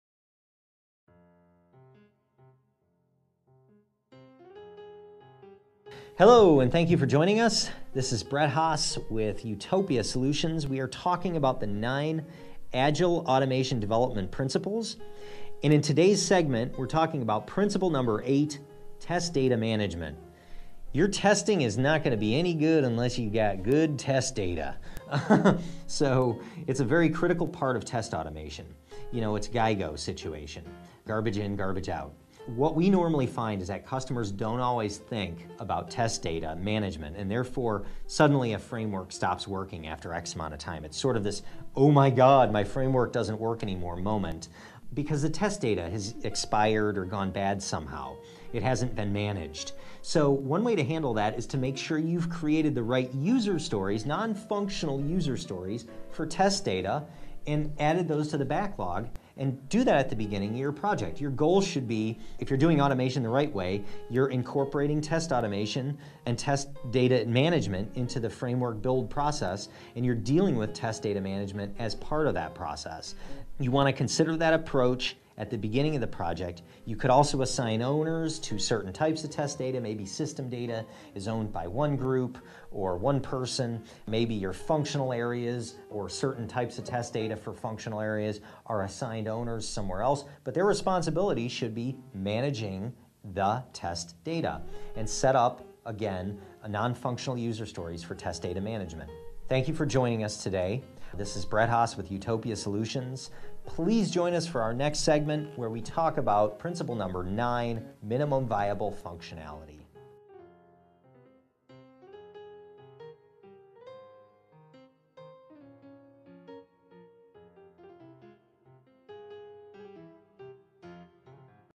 9 Agile Principles for Test Framework Development - #8 Test Data Management
In our eighth segment: Test Data Management Utopia Senior Test Automation Expert Brett Haase explains why this principle is so very important to successful framework development.

Hello, and thank you for joining us. This is Brett Haase with Utopia Solutions. We are talking about the Agile Automation Principles. And in today's segment, we are talking about Principle #8: Test Data Management. Your testing is not going to be any good unless you got good test data, so it's a very critical part of test automation. It's GIGO situation, garbage in, garbage out.

What we normally find is that customers don't always think about test data management, and therefore, suddenly a framework stops working after X amount of time, it's sort of this, "Oh my god, my framework doesn't work anymore" moment, because the test data has expired or gone bad somehow. It hasn't been managed.

So one way to handle that is to make sure that you've created the right user stories, non-functional user stories, for test data, and added those to the backlog, and do that at the beginning of your project. Your goal should be, if you're doing automation the right way, you're incorporating test automation and test data management into the framework build process, and you're dealing with test data management as part of that process.

You want to consider that approach at the beginning of the project. You can also assign owners to certain types of test data, maybe system data is owned by one group or one person. Maybe your functional areas or certain types of test data for functional areas are assigned owners somewhere else, but their responsibility should be managing the test data. And set up, again, non-functional user stories for test data management.

Thank you for joining us today. This is Brett Haase for Utopia Solutions. Please join us for our next segment where we talk about Principle #9, Minimum Viable Functionality.

In case you missed them here are the other videos in this series.